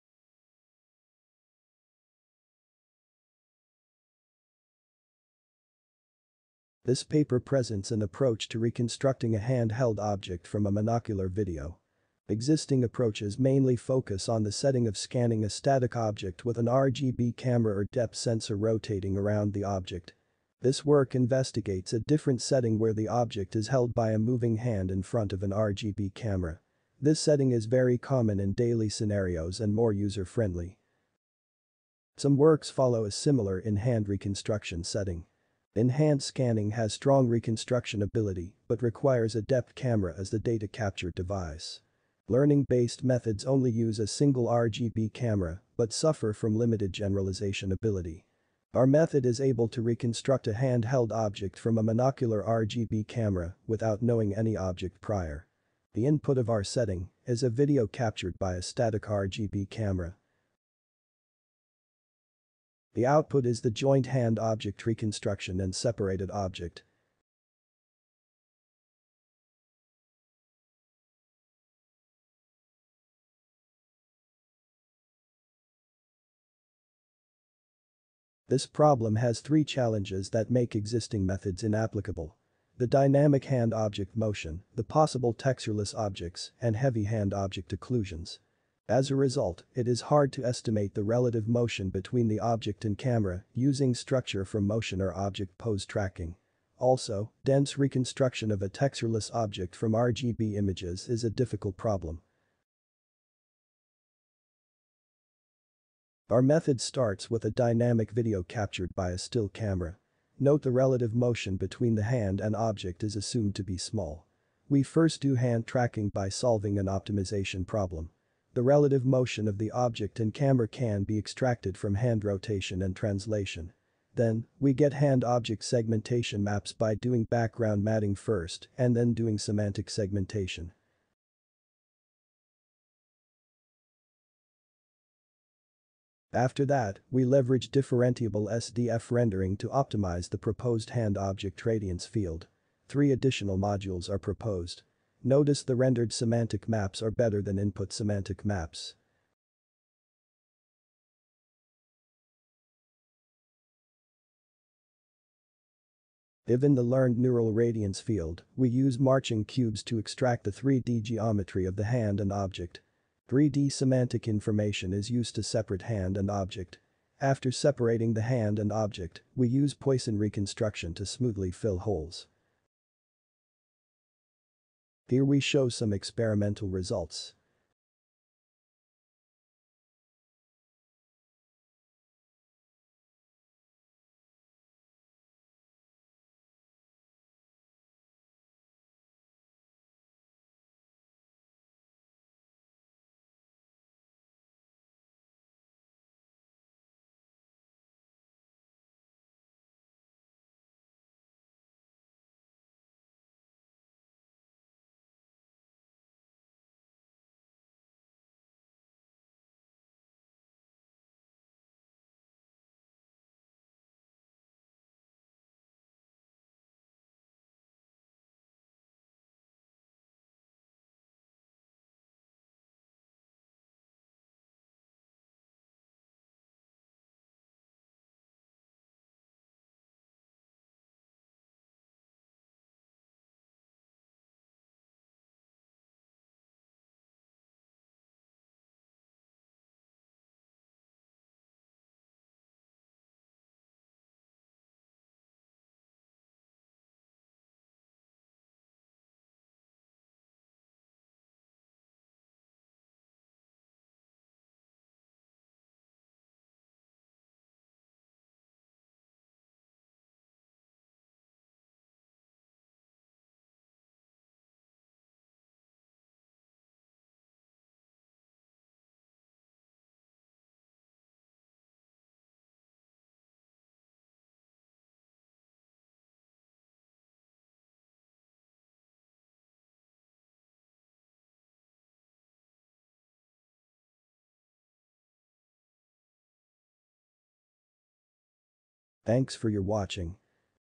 Reconstructing Hand-Held Objects from Monocular Video
This paper presents an approach that reconstructs a hand-held object from a monocular video. In contrast to many recent methods that directly predict object geometry by a trained network, the proposed approach does not require any learned prior about the object and is able to recover more accurate and detailed object geometry. The key idea is that the hand motion naturally provides multiple views of the object and the motion can be reliably estimated by a hand pose tracker. Then, the object geometry can be recovered by solving a multi-view reconstruction problem. We devise an implicit neural representation-based method to solve the reconstruction problem and address the issues of imprecise hand pose estimation, relative hand-object motion, and insufficient geometry optimization for small objects. We also provide a newly collected dataset with 3D ground truth to validate the proposed approach.